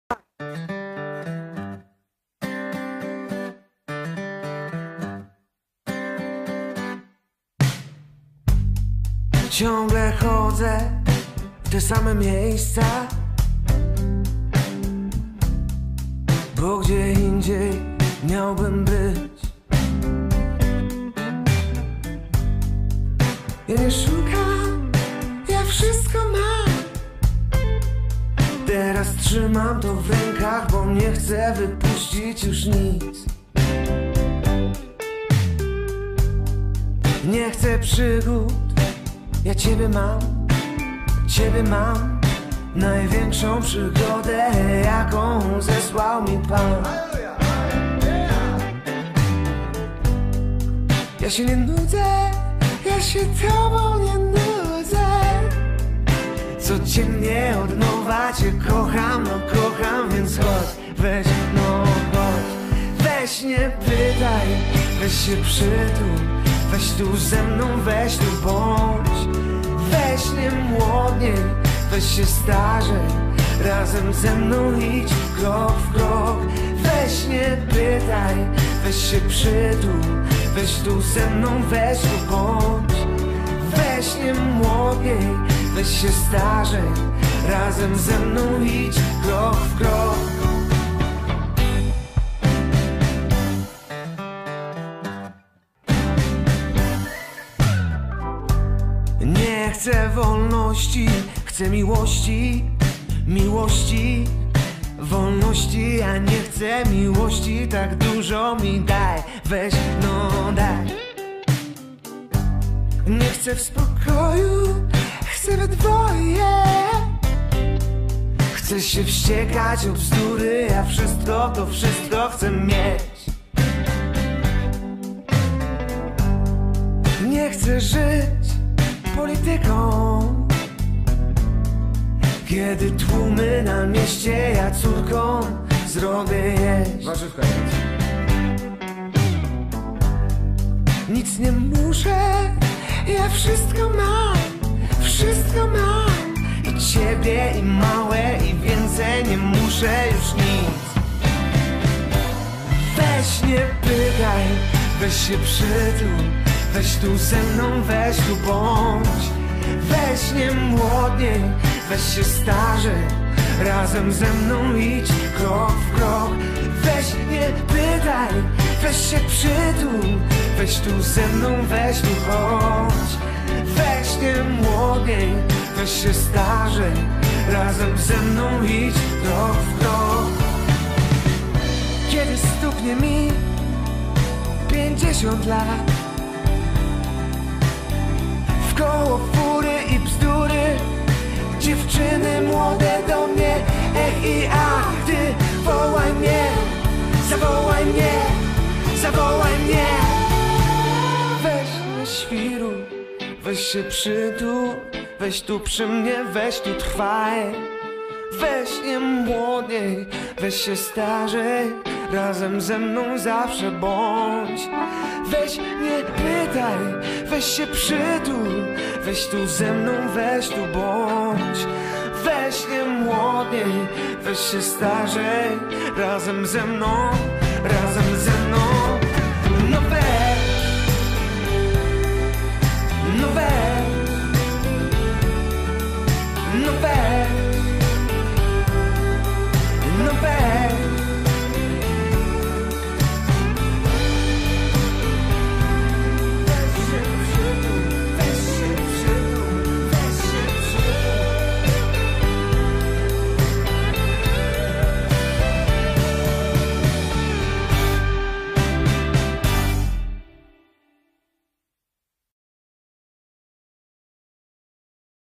XVI Ognisko Integracyjne
Relacja z Ogniska Integracyjnego, zorganizowanego przez 6 Dom Pomocy Społecznej w Ośrodku Edukacji Ekologicznej w dn. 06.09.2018 r.